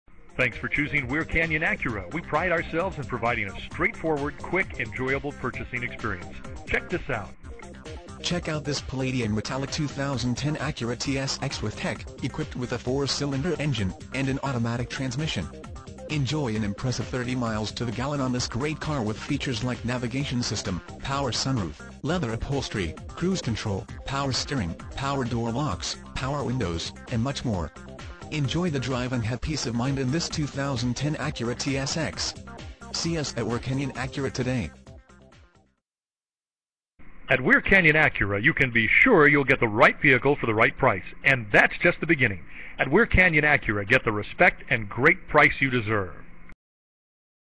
Acura TSX, Weir Canyon Acura- Anaheim, CA 92807
Acura TSX, Weir Canyon Acura imagine driving this Palladium Metallic 2010 Acura TSX, equipped with a 4 Cyl. engine and an automatic transmission with only 20,491 miles. enjoy an impressive 30 miles to the gallon on this great car with features like Navigation System, Power Sunroof, Leather Upholstery, Cruise Control, Power Steering, Power Door Locks, Power Windows, and much more. Enjoy the drive and have peace of mind in this 2010 Acura TSX. see us at Weir Canyon Acura today!
miles-  20,491
vin- JH4CU2F64AC029427

8323 E La Palma
Anaheim, California 92807